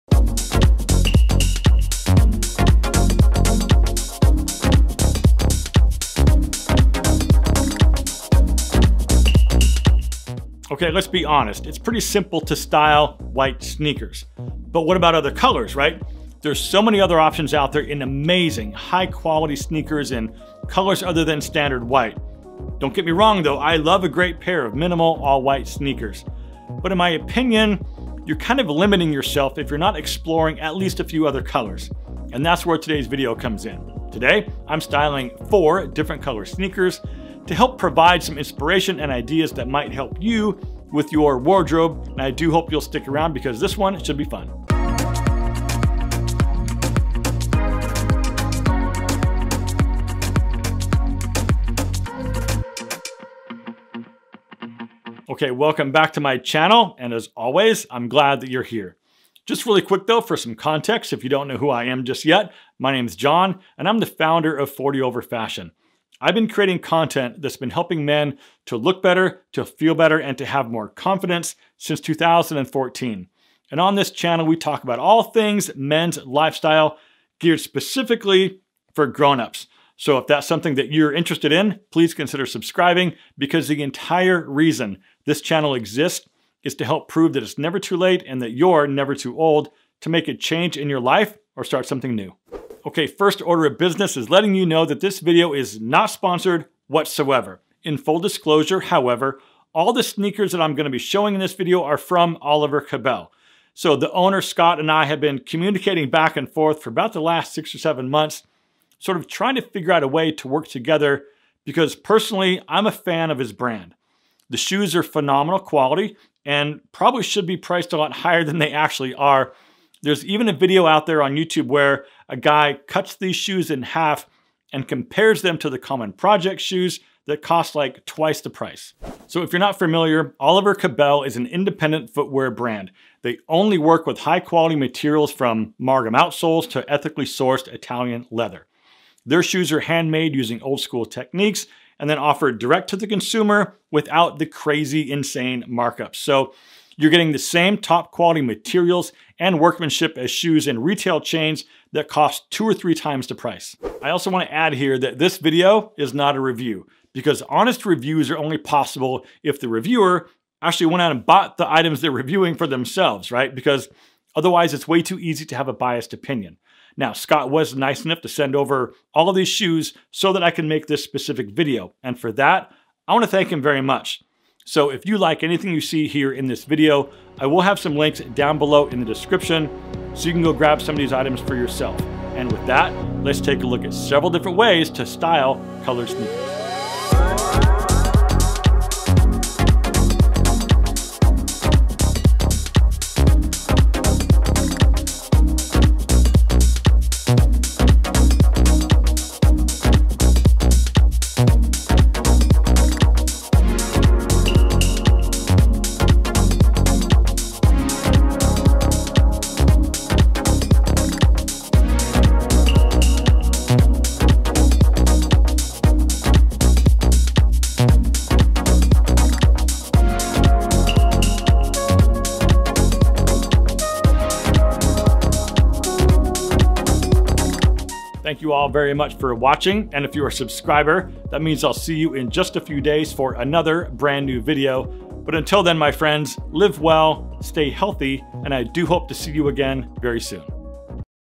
How To Style Colored Sneakers For Men | Non-White Sneakers
Today I'm showing several different ways you can style colored, non-white sneakers.
*Some of the pieces I'm wearing in this video:
Express Chinos
American Eagle Jeans

\\ 40 Over Fashion AMAZON STORE!